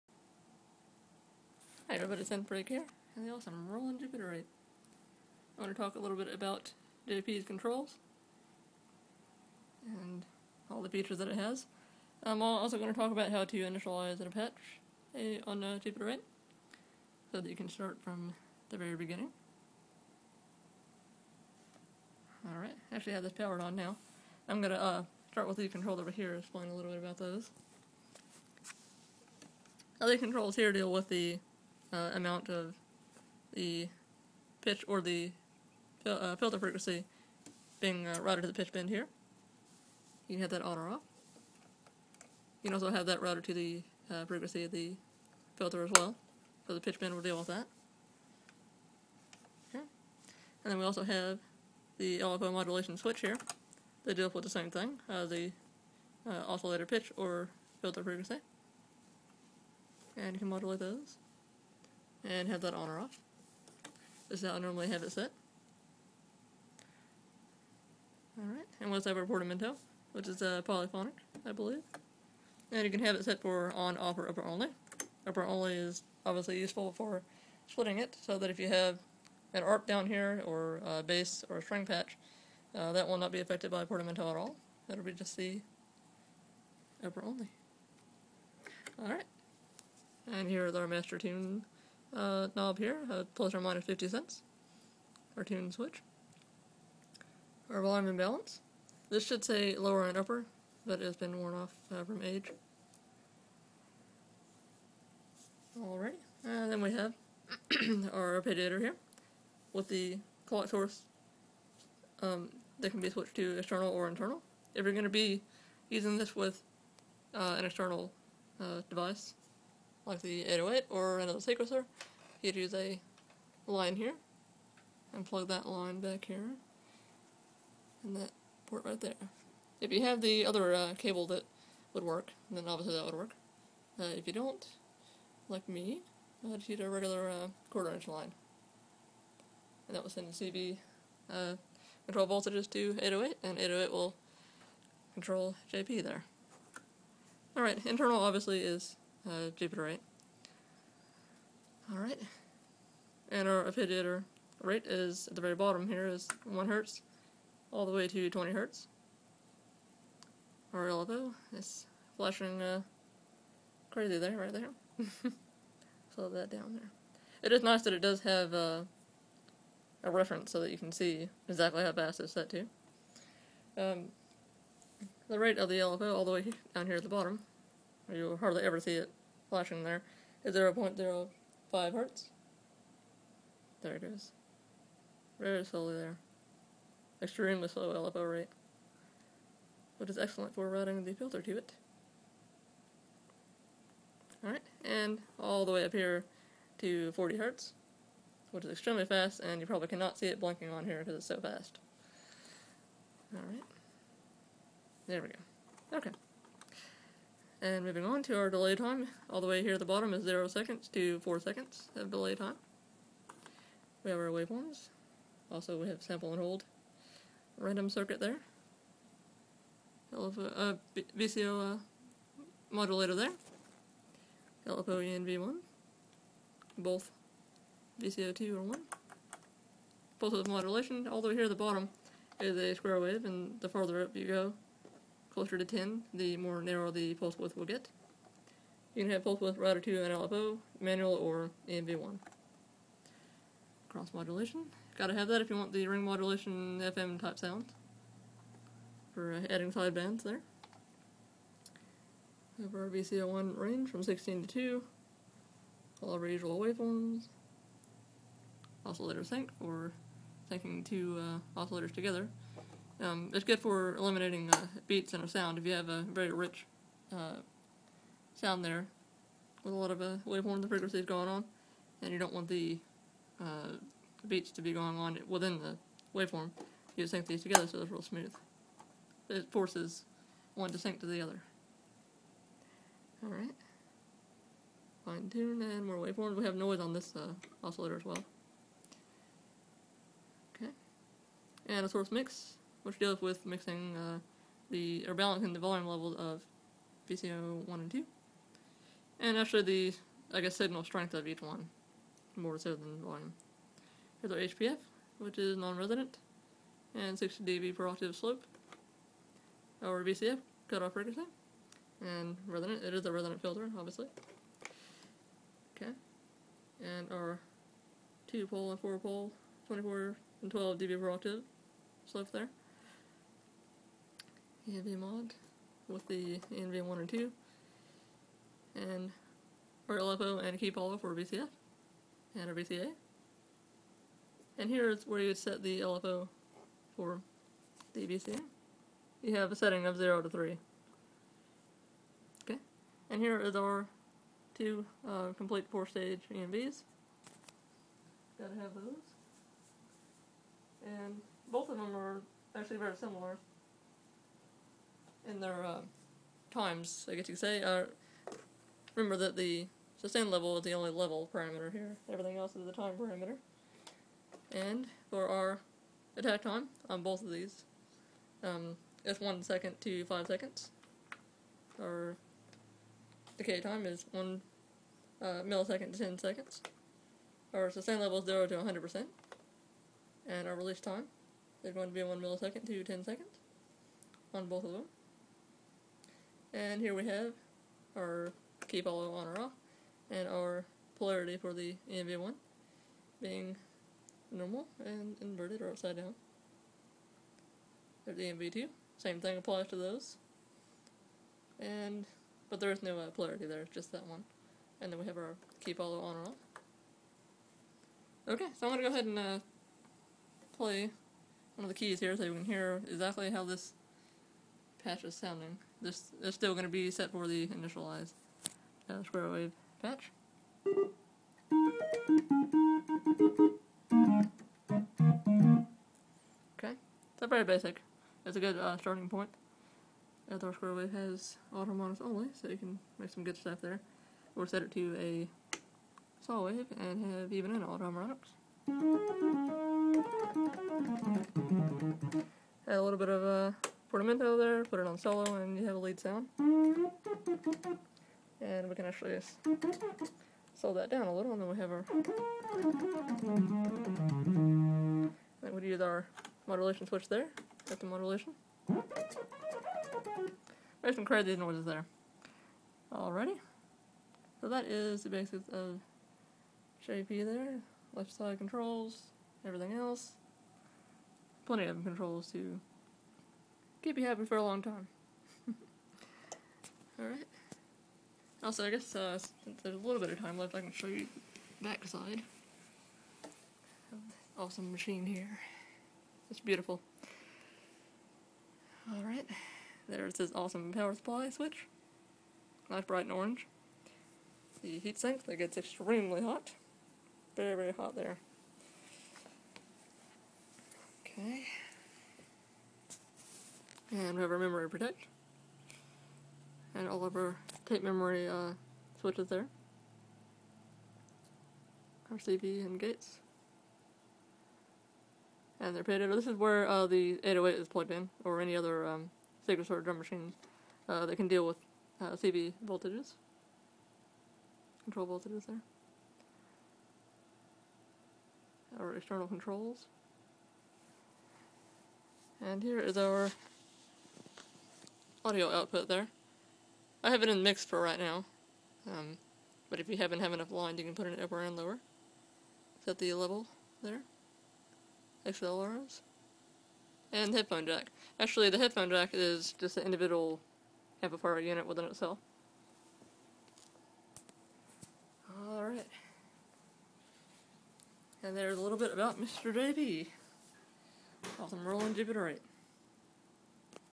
Jupiter-8: overview, controls, init patch.
Roland Jupiter-8: 8 voice, 16 osc polyphonic analog synthesizer. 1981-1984. The most beautiful synth ever. :)